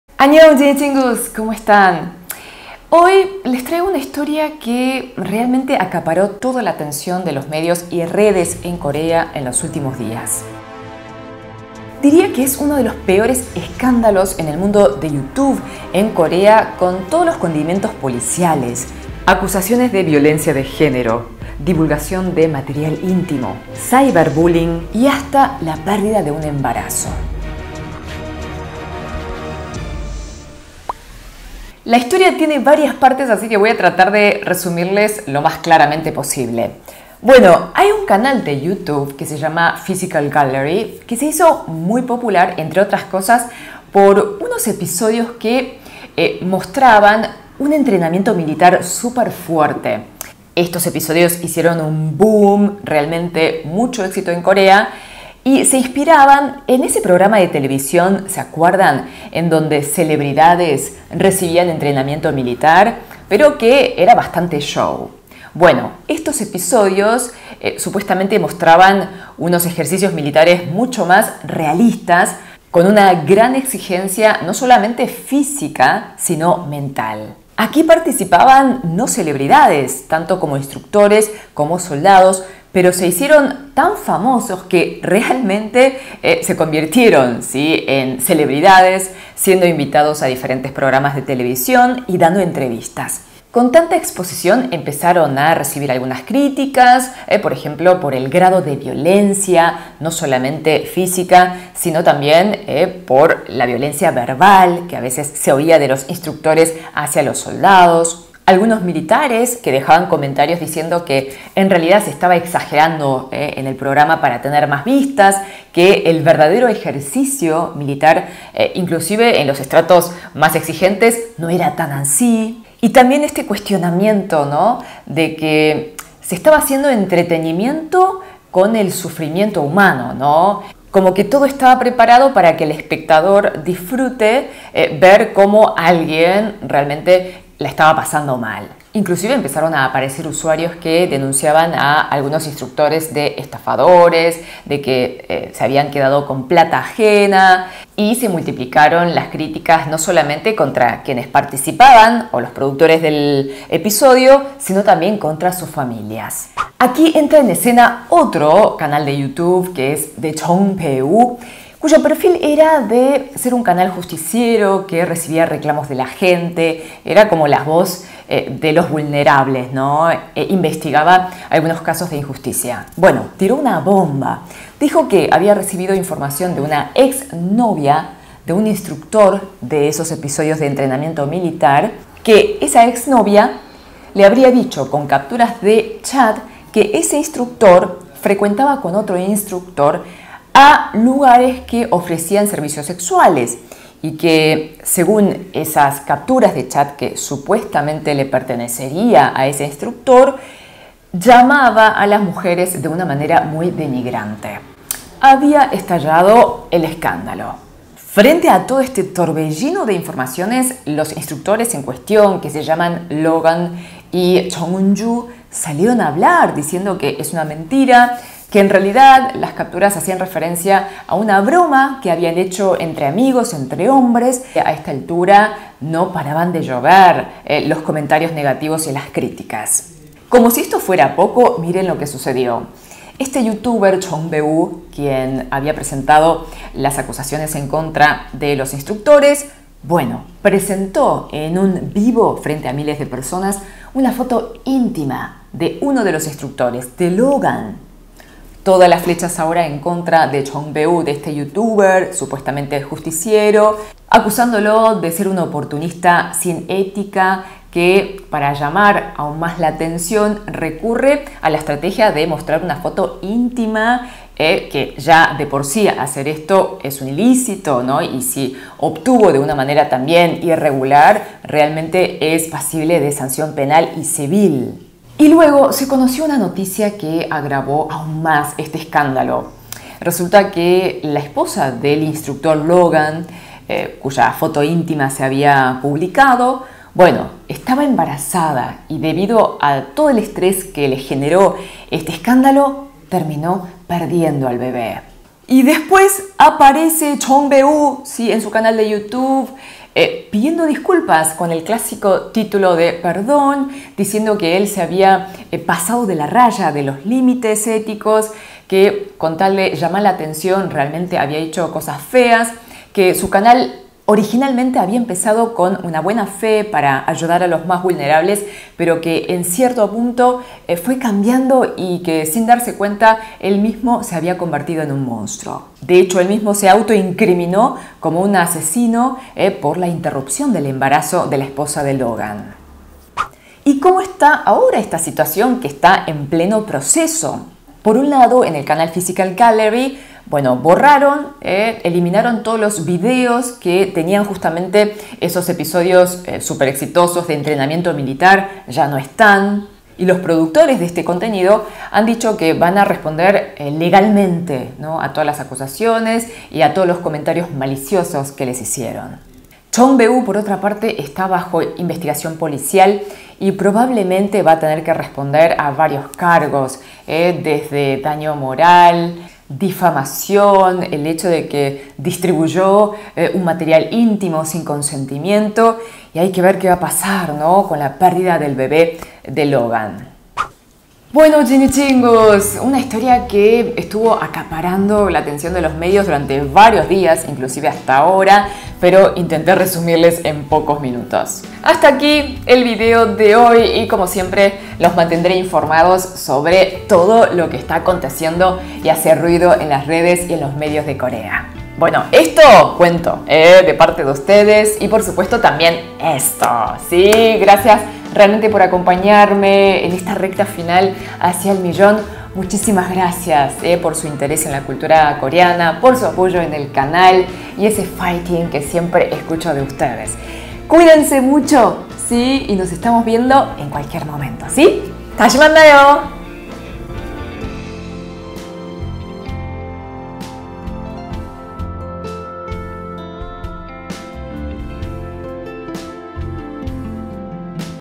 EL PEOR ESCÁNDALO DE YOUTUBE EN COREA. DOS DE LOS CANALES MÁS POPULARES EN PROBLEMAS.
¡Anñong JiniChingus! Hoy les traigo una noticia que está sonando muchísimo en las redes y en los medios coreanos. Se trata de un escándalo que tienen todos los ribetes de una nota policial: acusaciones de violencia de género, ciberbullying, falta de ética y se involucra hasta una tragedia.

✔ JiniTrend Instagram (KBeauty y KFashion)